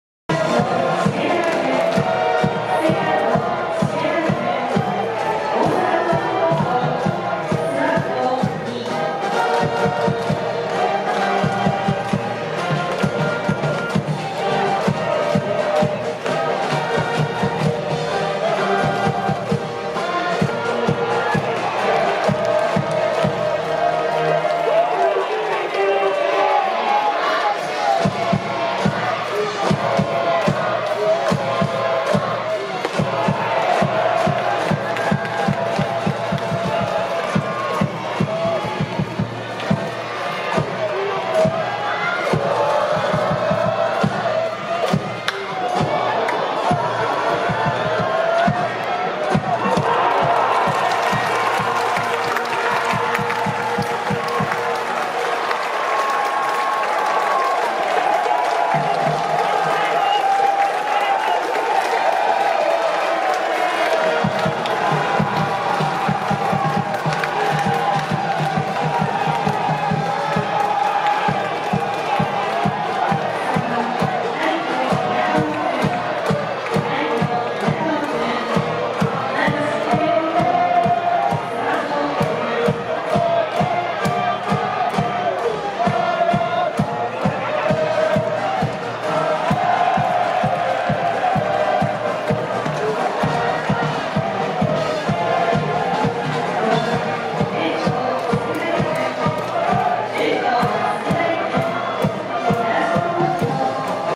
繁永 晟【先制ホームラン】中大3年 大阪桐蔭 2024.4.8 vs駒大 神宮球場
2024年4年8日 東都大学野球春季リーグ戦 中央大学vs駒澤大学